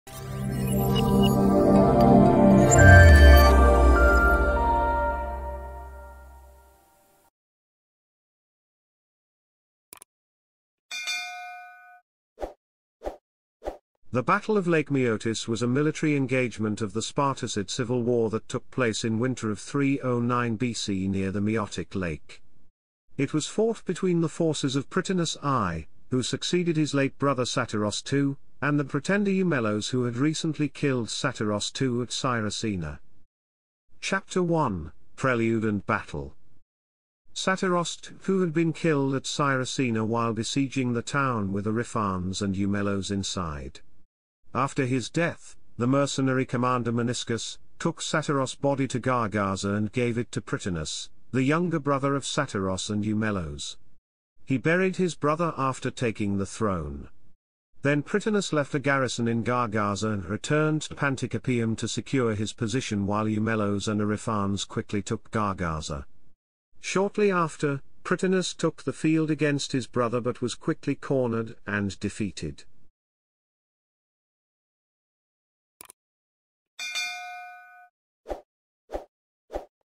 Battle of Lake Maeotis - Wikipedia article
Video summary:
The Battle of Lake Maeotis was a military engagement of the Spartocid civil war that took place in winter of 309 BC near the Maeotic Lake. It was fought between the forces of Prytanis I, who succeeded his late brother Satyros II, and the pretender Eumelos who had recently killed Satyros II at Siracena.